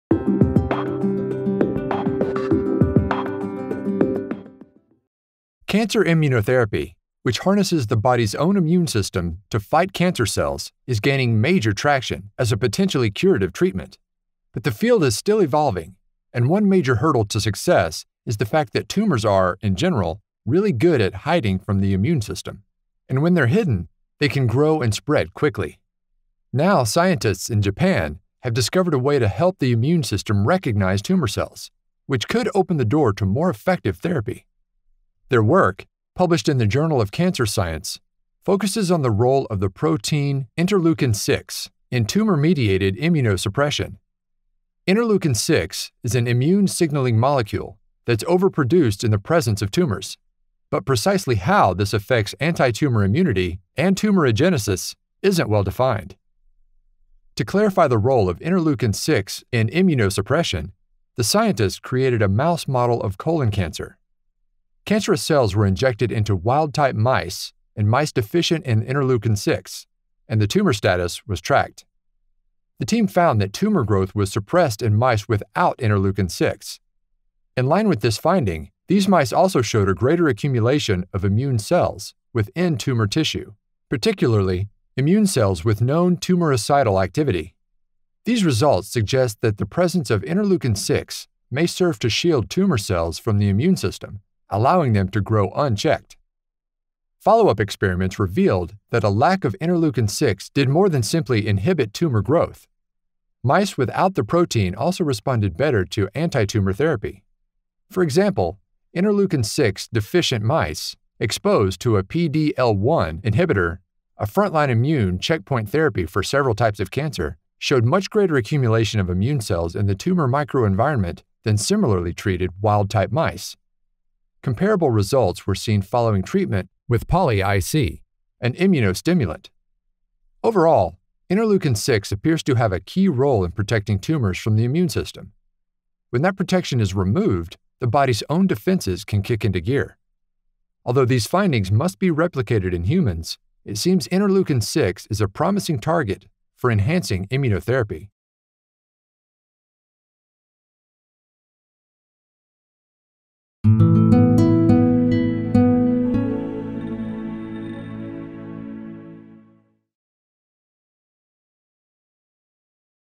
Interleukin-6 may shield tumors from the immune system, hindering treatment efforts
Video abstract on the Cancer Science article "Lack of interleukin-6 in the tumor microenvironment augments type-1 immunity and increases the efficacy of cancer immunotherapy" by Ohno et al.